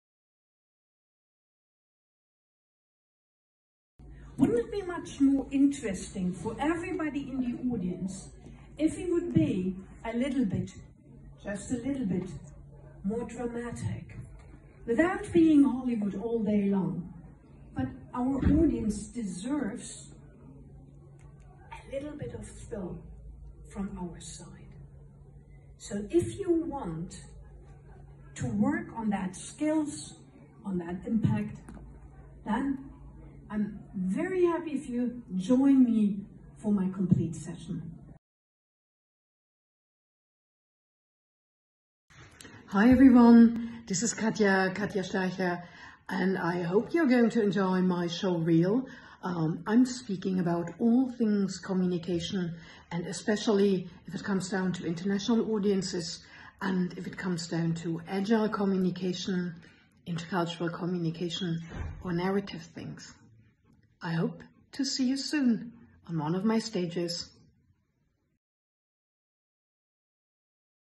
Speaker Showreel Katja Schleicher 2023 english
"I get you talking"
Speaking professional Katja Schleicher 2023
Katja is a as a speaker fun and no-nonsense combined. What she points out about communication, is helping her audiences to speak up ans out more easily, honestly and impactful.